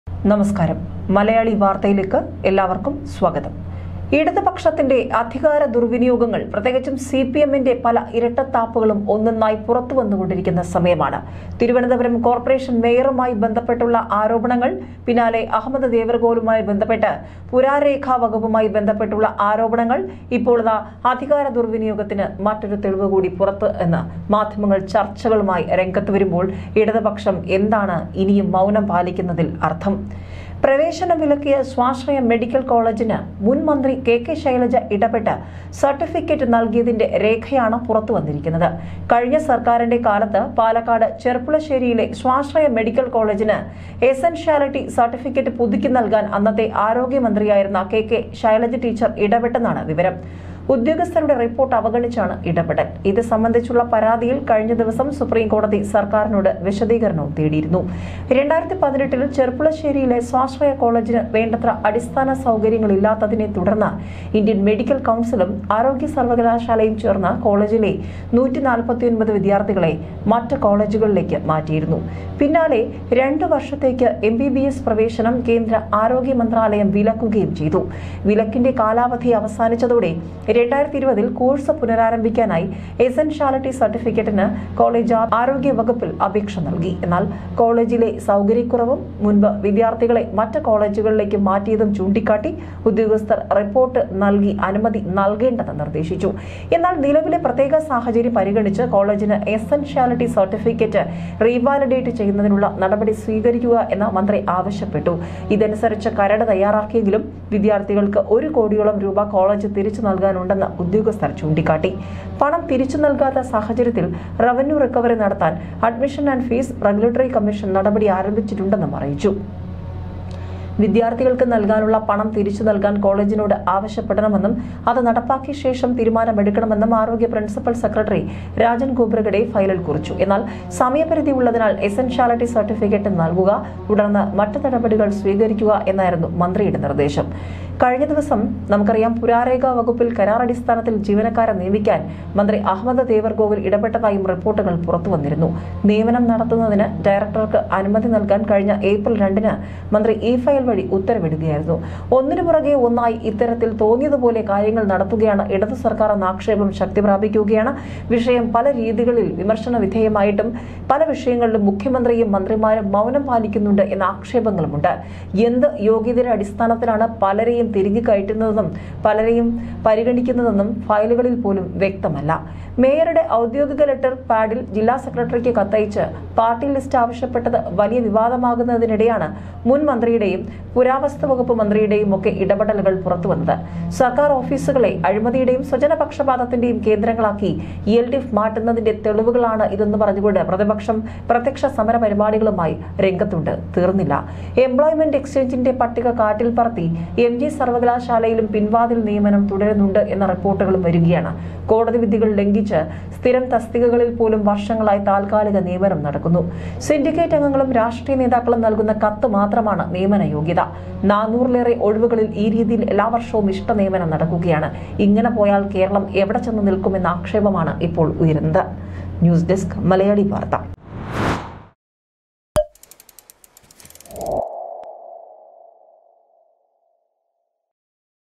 "K K Shylaja ടീച്ചറമ്മയും നഗരകമാതാവിന് പിന്നാലെ"
" 'വ്യാജ' സര്‍ട്ടിഫിക്കറ്റും പുറത്ത്?
'മുന്‍മന്ത്രിക്കും മന്ത്രിക്കും പണി.